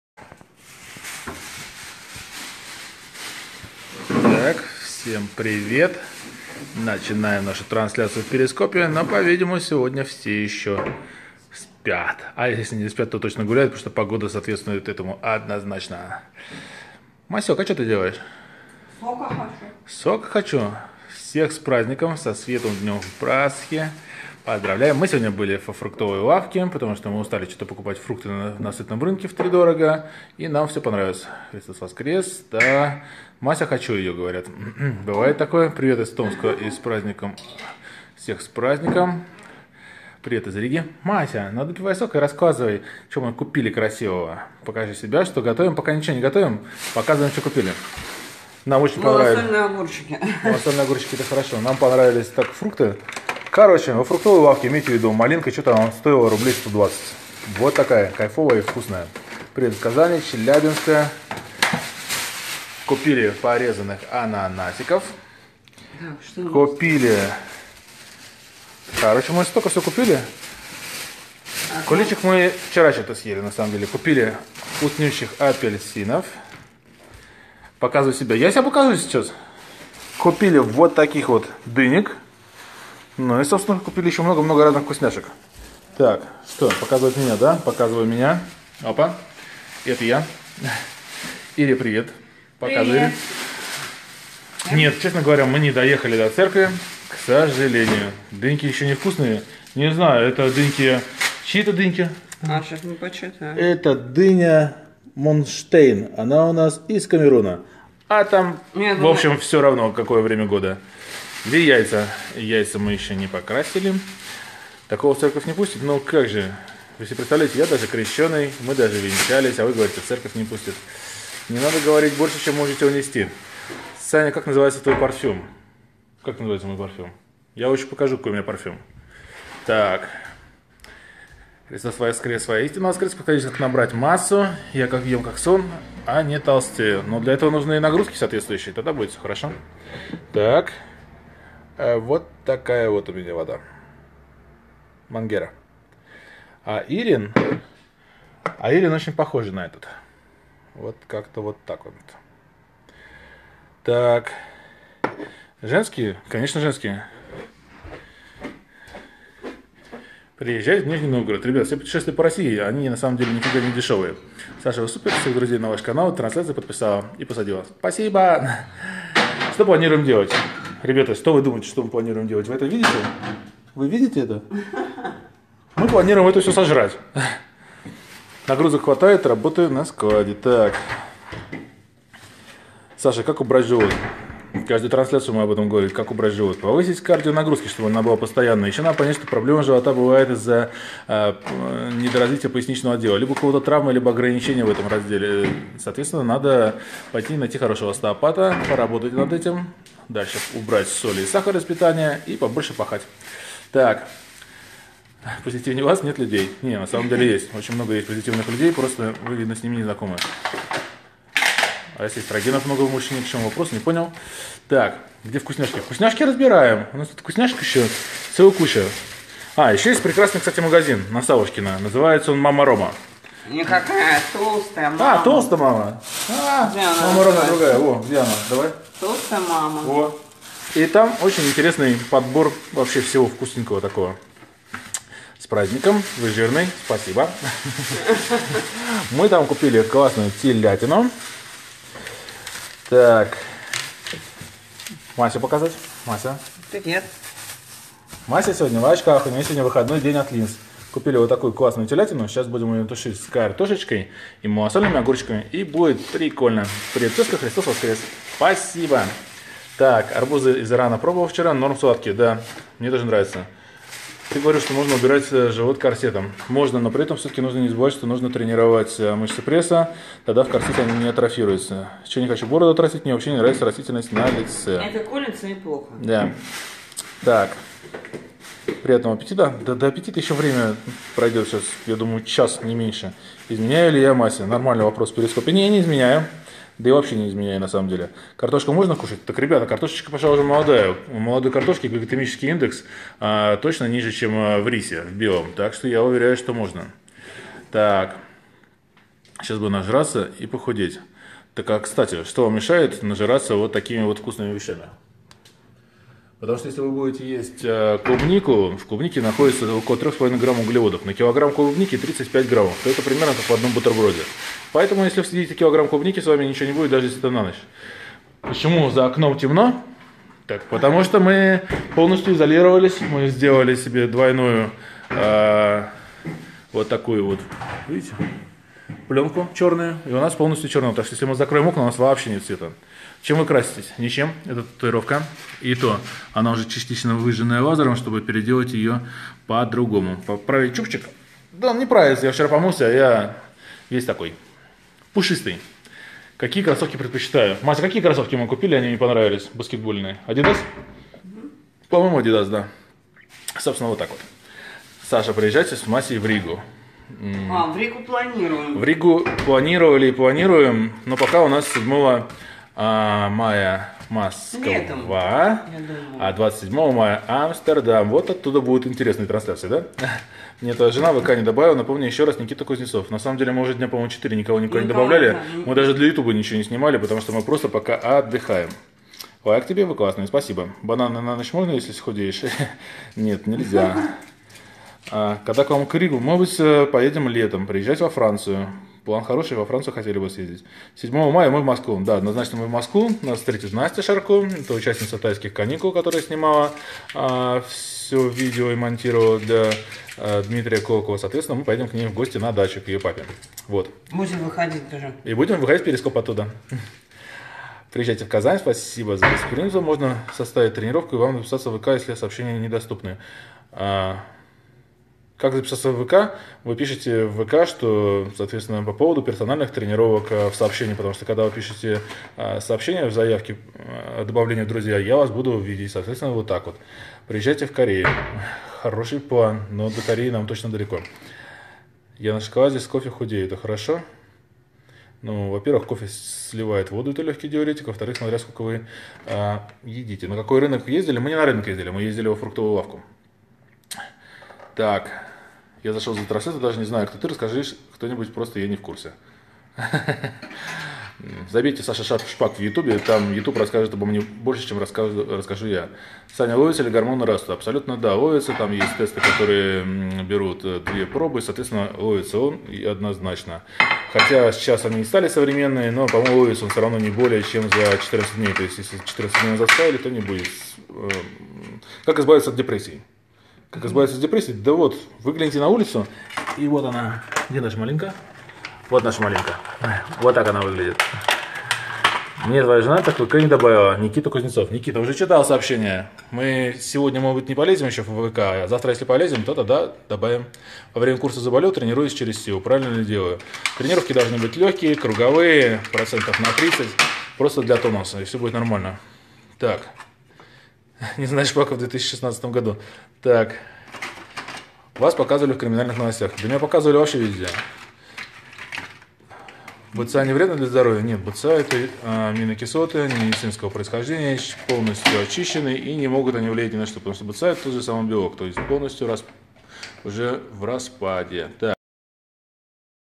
С днём ПАСХИ братья и сестры!!!
Александр Шпак поздравляет с Пасхой!!!